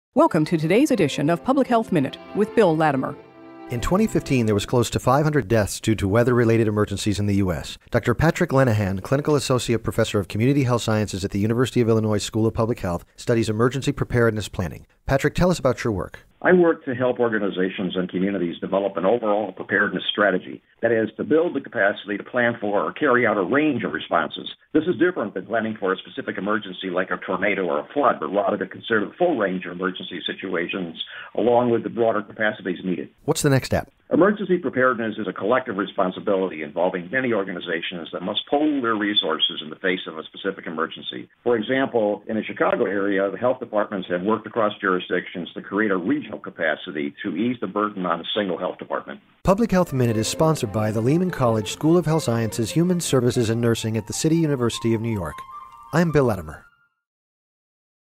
Public Health Minute with Dr. William Latimer: Dr. Patrick Lenihan
Dr. Patrick Lenihan, Clinical Associate Professor at the University of Illinois School of Public Health, Director of the Doctor of Public Health in Leadership program, talks about emergency preparedness and planning.